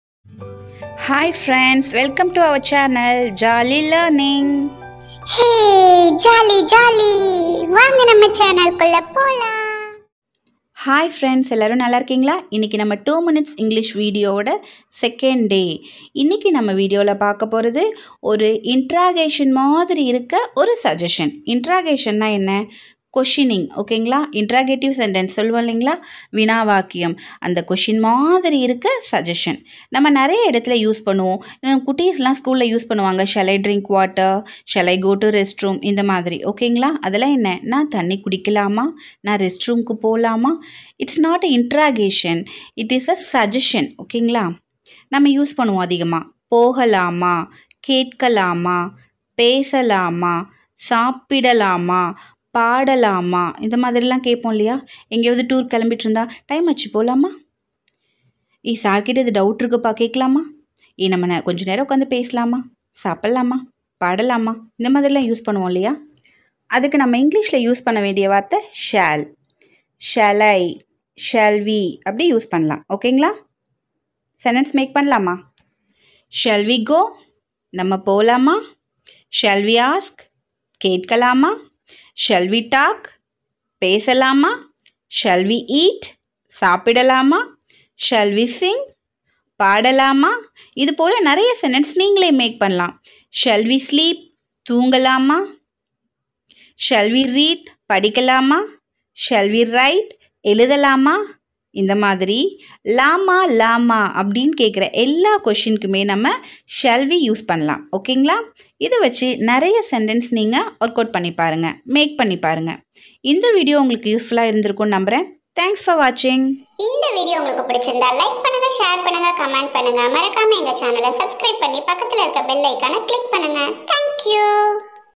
2 min English/Day 2/ Shall we...? / ... லாமா?/ usage of shall/Easy English/ speak English
This video gives you" usage of shall".

Have Jolly learning!!!

All contents provided by this channel for GENERAL AND EDUCATIONAL PURPOSE ONLY.